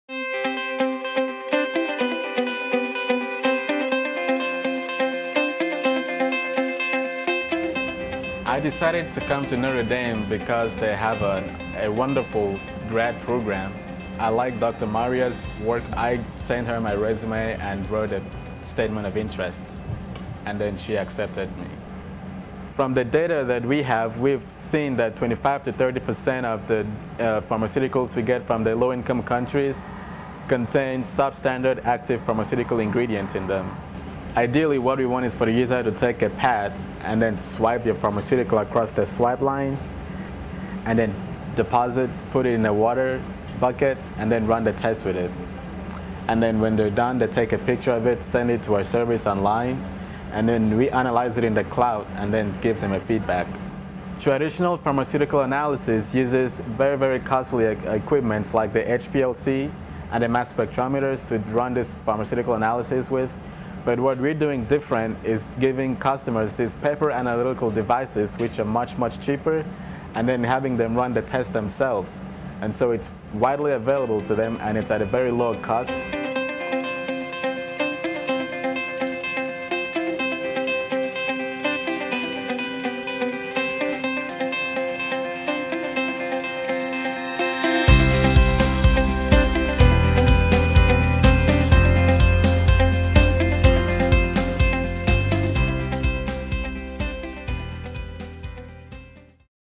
Undergraduate Research Experience: The PAD Project
Winthrop University undergraduate Esse Etim spent summer 2014 at the University of Notre Dame doing research with Marya Lieberman, associate professor of chemistry and biochemistry. His research focused on working with paper test cards to analyze potential counterfeit pharmaceuticals.